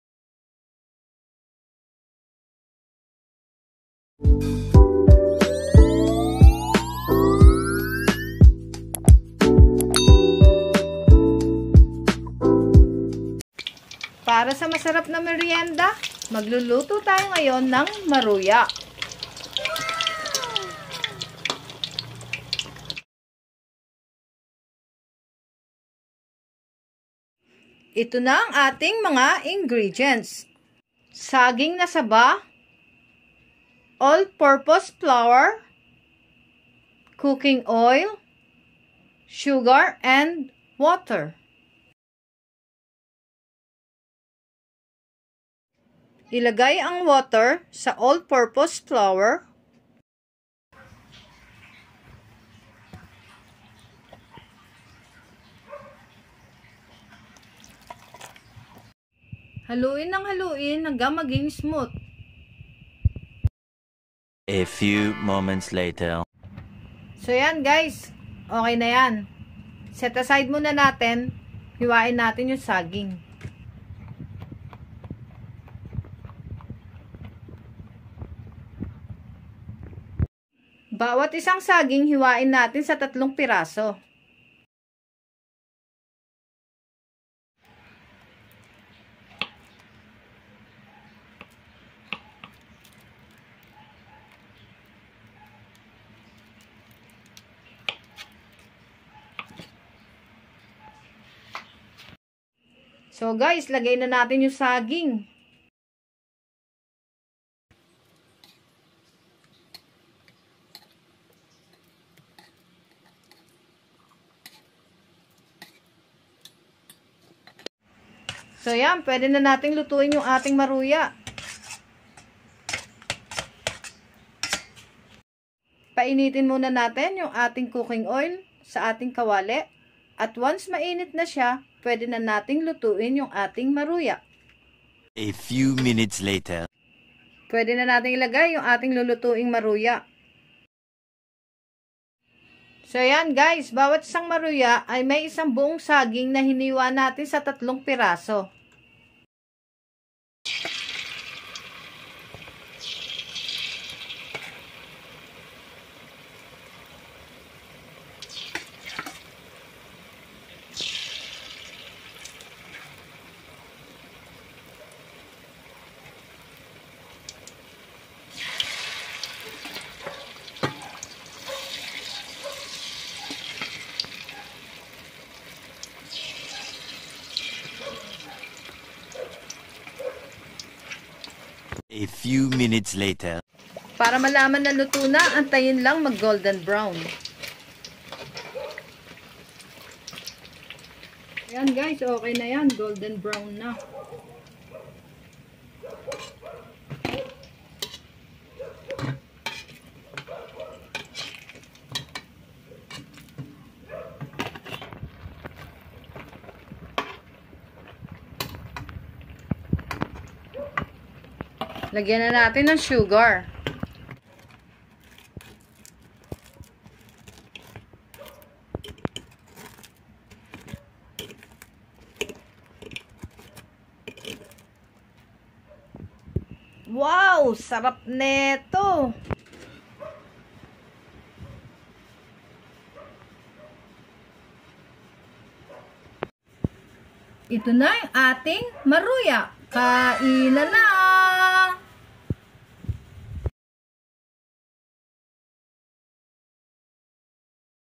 HOW TO COOK MARUYA
INGREDIENTS:

5 pcs Saging na Saba
1 cup all purpose flour
1/2 cup sugar
1 cup water
1 and 1/2 cup cooking oil